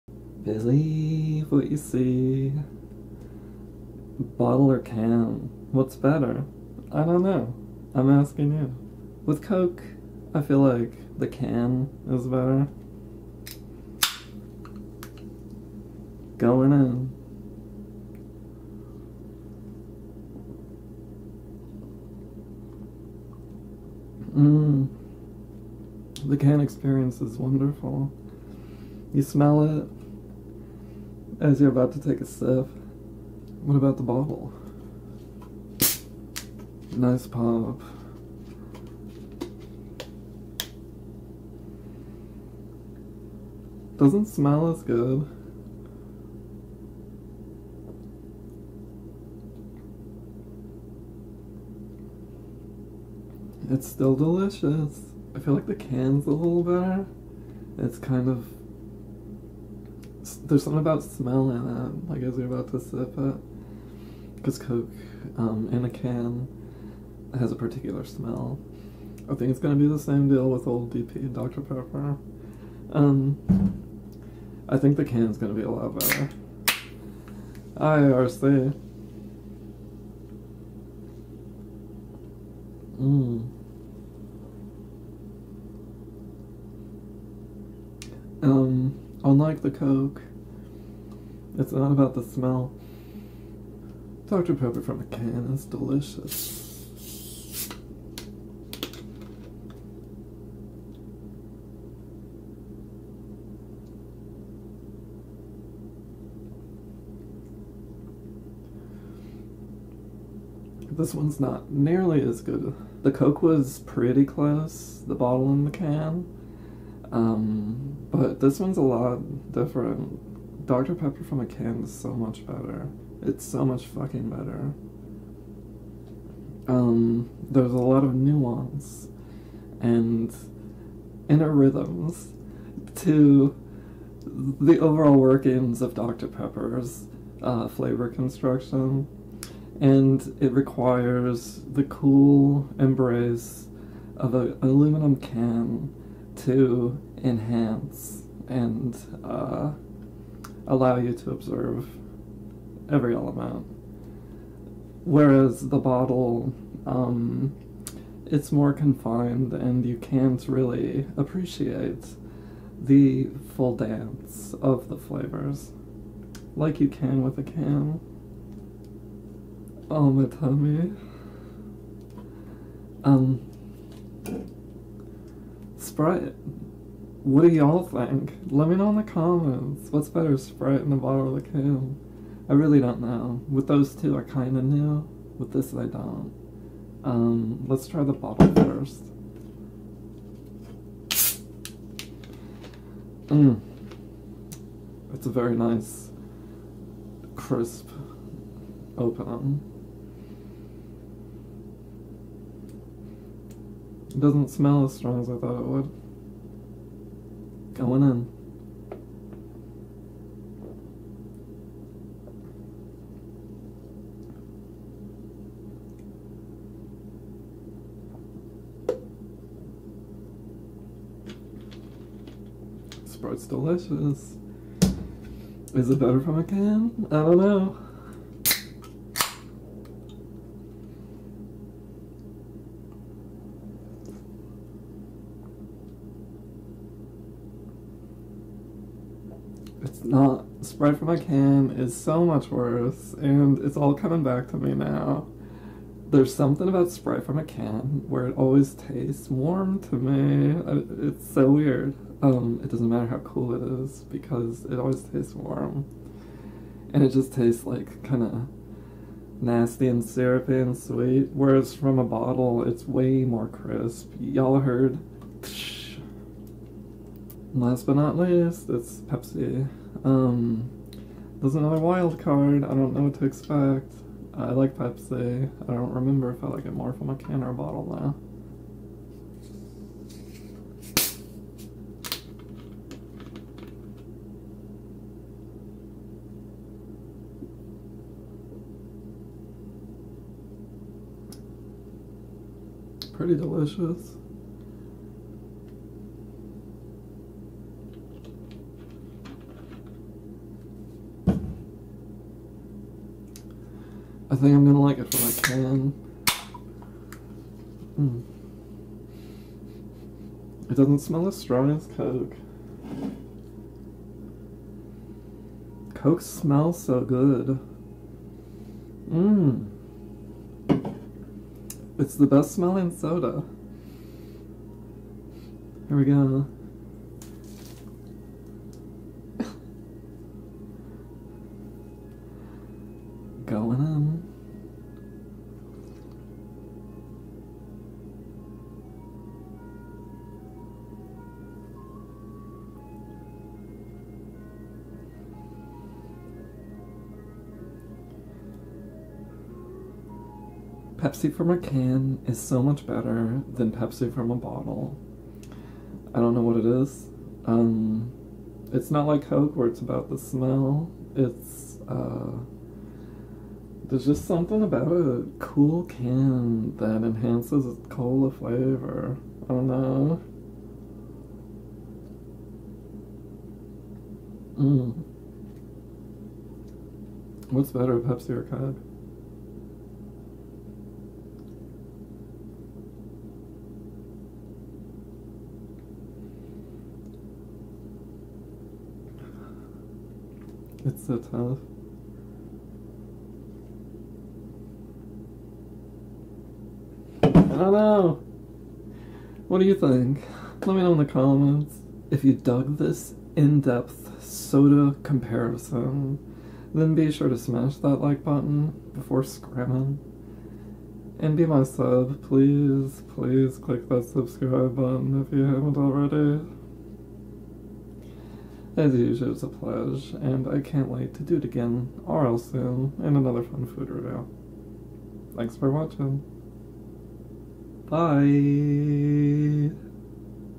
Does soda taste better from a bottle or a can??
Coke vs Pepsi vs Dr Pepper vs Sprite...when is a can or a bottle better? LMK in the comments!